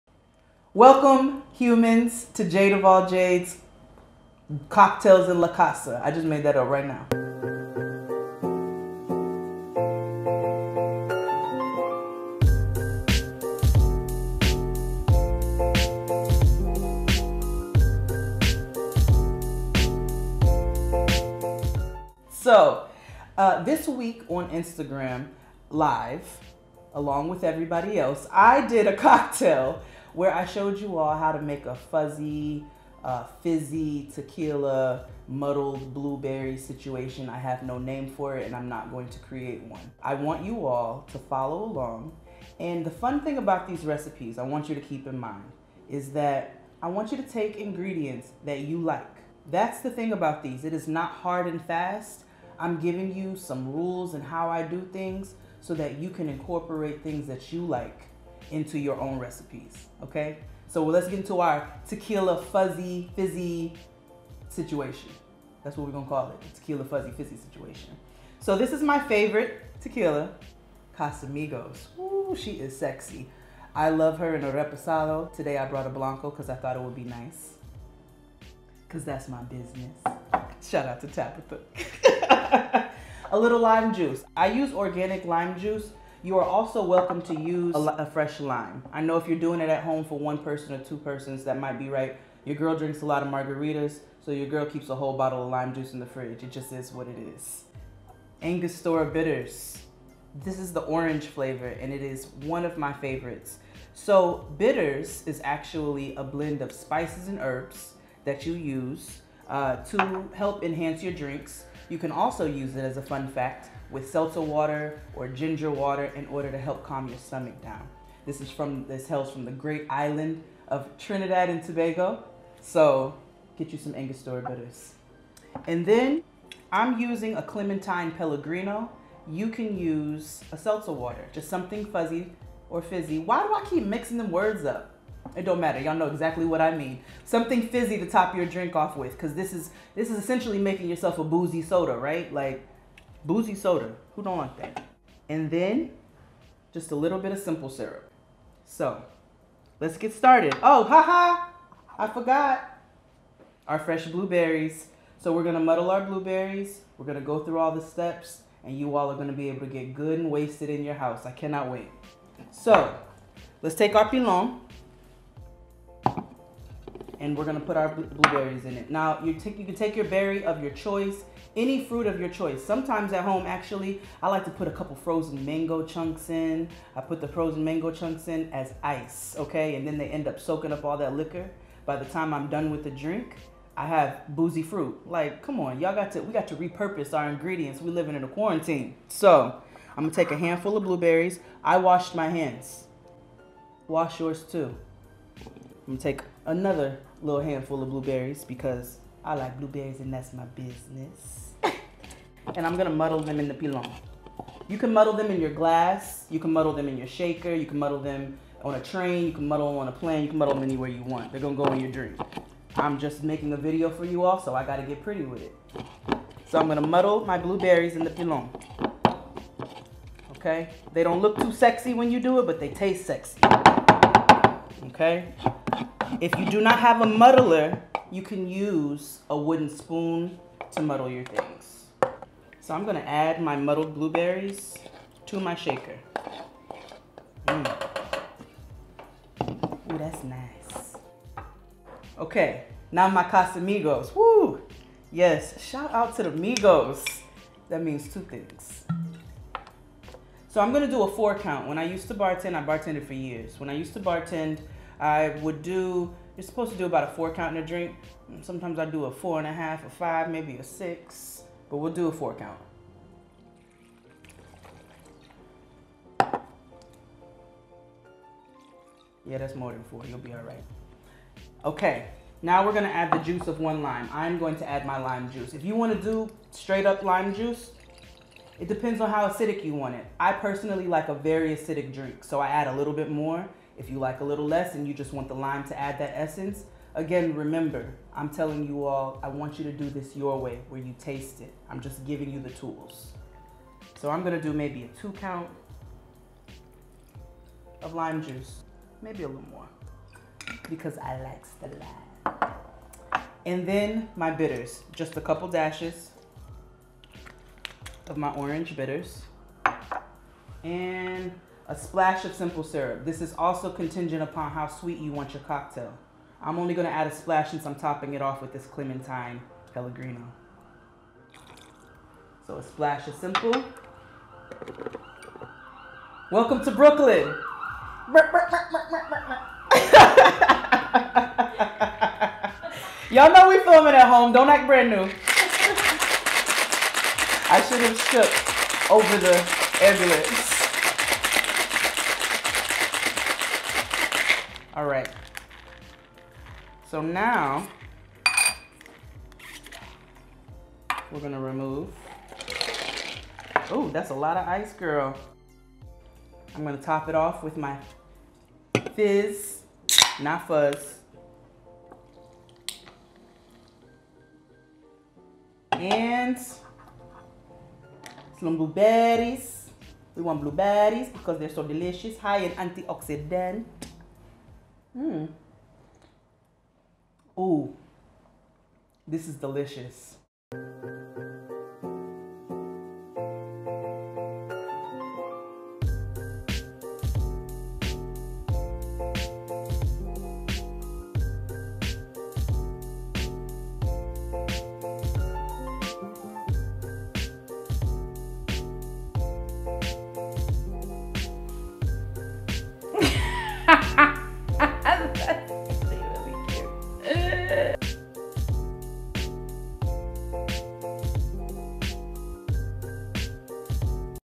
Brooklyn Blueberry Fuzzy | Cocktails En La Casa with Jade - EP1
On Fridays, we make cocktails. Reward yourself with this delicious Brooklyn Blueberry Fuzzy cocktail en la casa with @jadeofalljades.